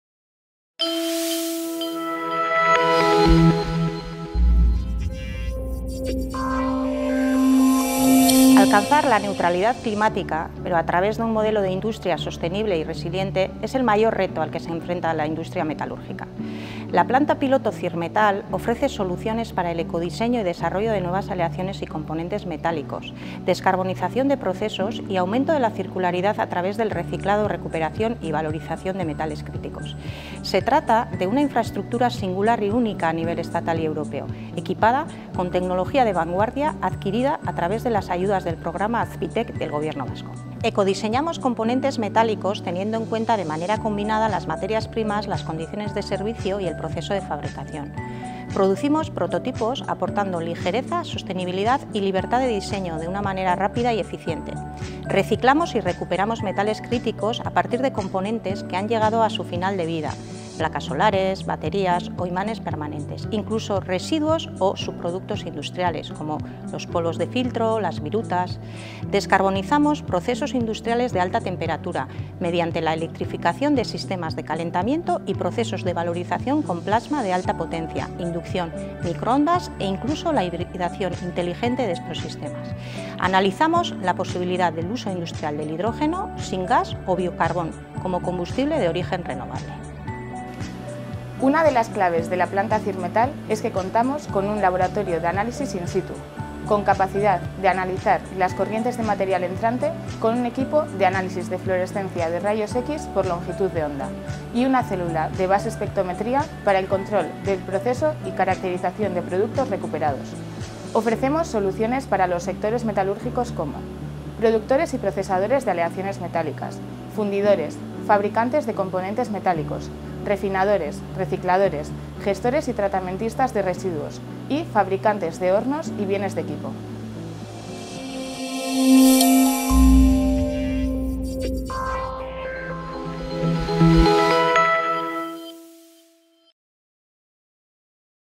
Planta Piloto CIRMETAL: investigación hacia una MetalurgiaSostenible y CircularidadMetálica
CIRMETAL ofrece soluciones para el ecodiseño y desarrollo de nuevas aleaciones y componentes metálicos, descarbonización de procesos y aumento de la circularidad de materiales a través del reciclado, recuperación y valorización de metales.

Persona de contacto
Dr. Maider García de Cortázar
Responsable Planta Piloto de metalurgia sostenible y circularidad : CIRMETAL